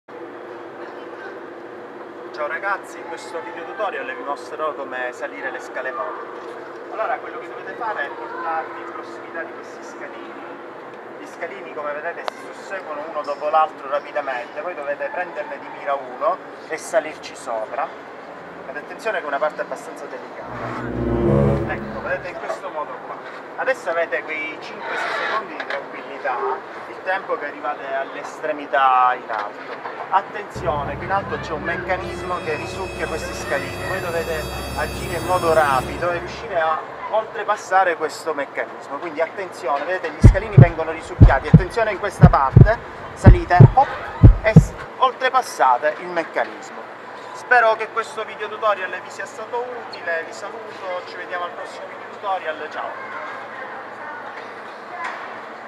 Tutorial - Come salire le scale mobili
Questo tutorial vi accompagnerà passo passo nella complessa avventura del salire le scale mobili.